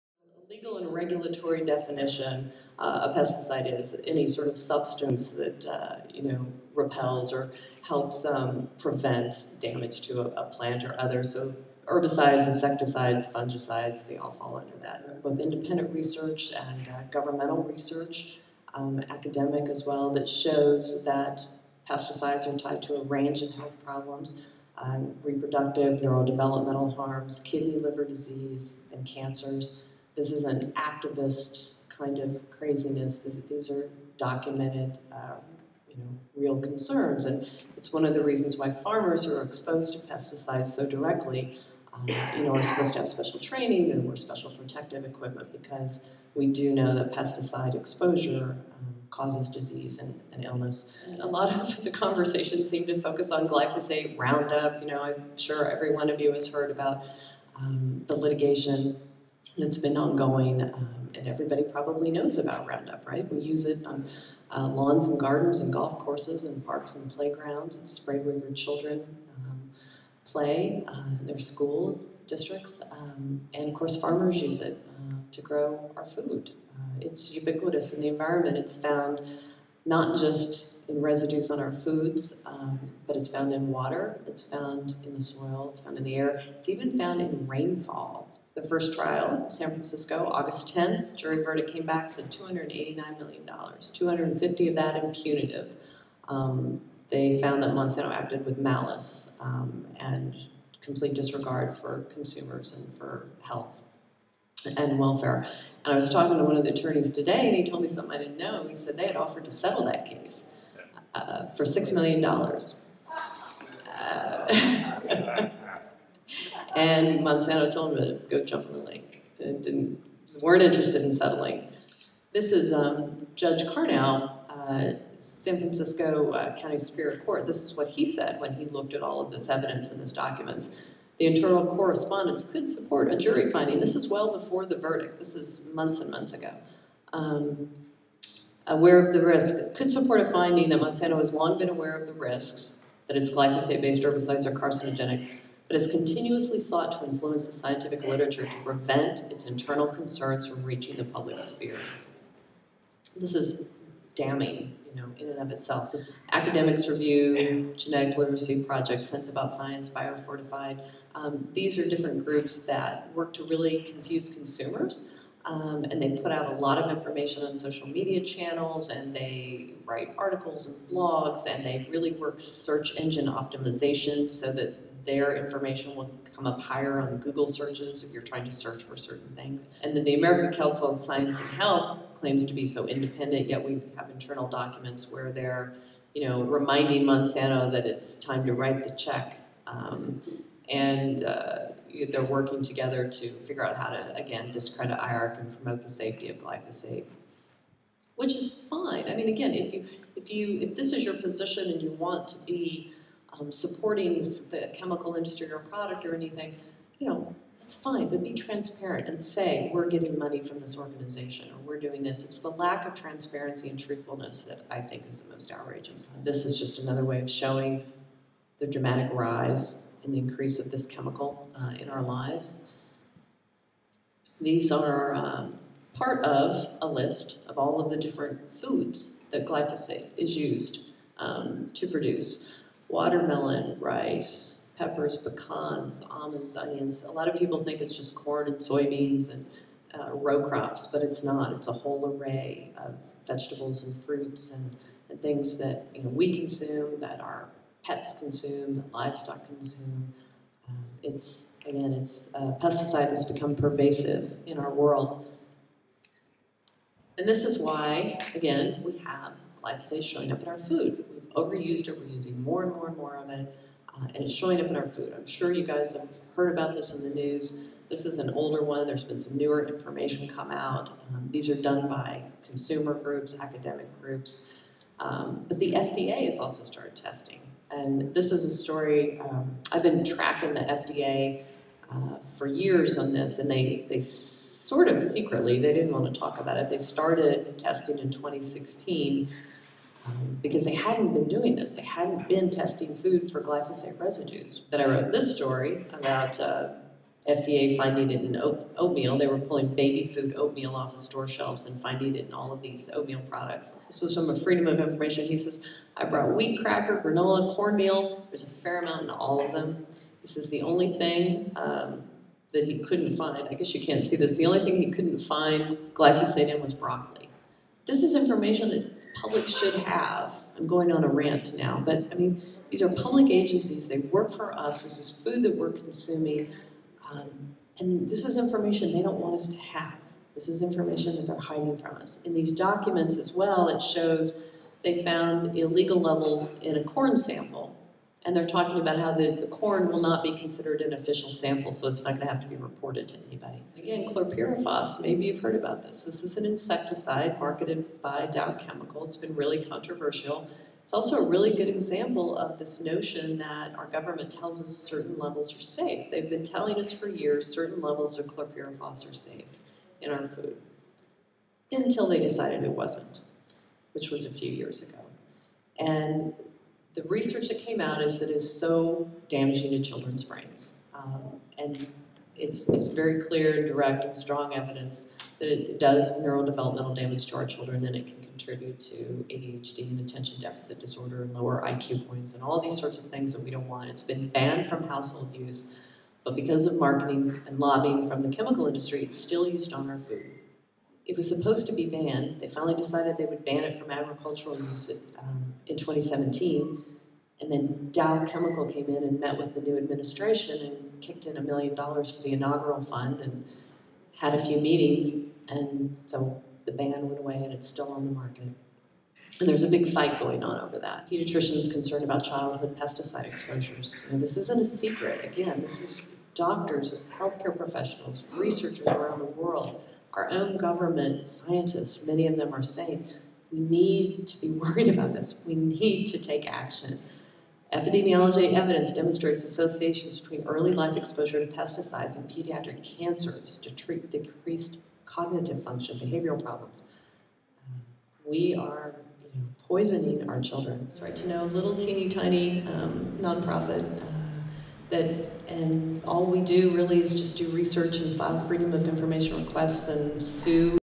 Soil Not Oil 2018 Tuesday Closing Keynote
The concurrence concludes with a highly insightful view of Monsanto by Investigative Journalist, Researcher and Writer Carey Gillam.

Presented at the Soil Not Oil 2018 Environmental Conference, San Francisco CA

Soil Not Oil promotes agro-ecological practices and community powered solutions to restore ecosystems and reach environmental justice.

Video by Paul Stelhe our amazing worktrade filmmakers, and Survival Media Agency